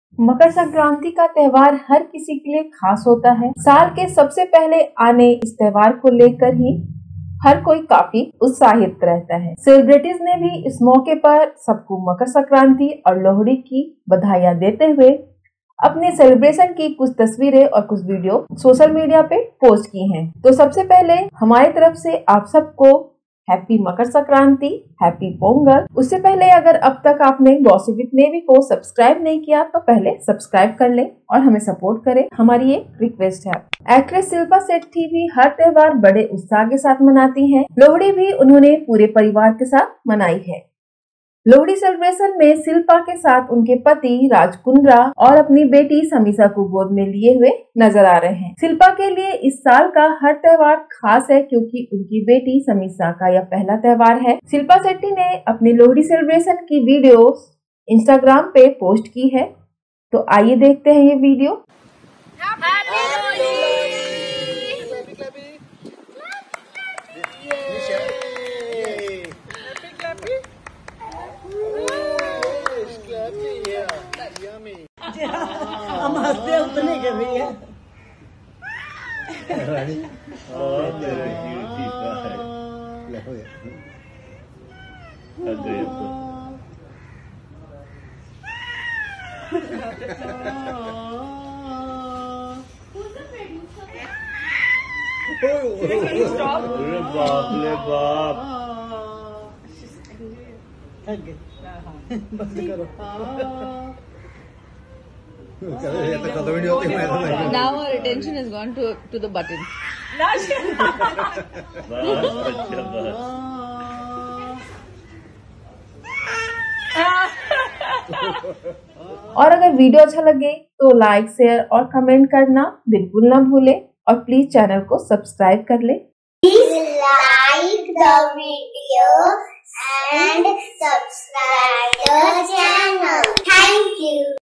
Shilpa Shetty Raj Kundra Celebrate First Lohri with daughter Samisha Shetty, Viaan and whole Family
Shilpa Shetty celebrated Lohri with her family in a traditional way on Wednesday, a glimpse of which she shared on her social media profile. The actress posted a photo from her celebrations that features her husband Raj Kundra, son Viaan, little daughter Samisha, mother Sunanda Shetty, father-in-law Bal Krishan Kundra and mother-in-law Usha Rani Kundra. Sharing the video, Shilpa Shetty wished her fans: "Lohri di lakh lakh vadhiyaan saareyaan nu. May the Lohri fire burn away all the negativity; and bring you joy, happiness, prosperity, and love. Wishing you and your loved ones a very Happy Lohri from our family."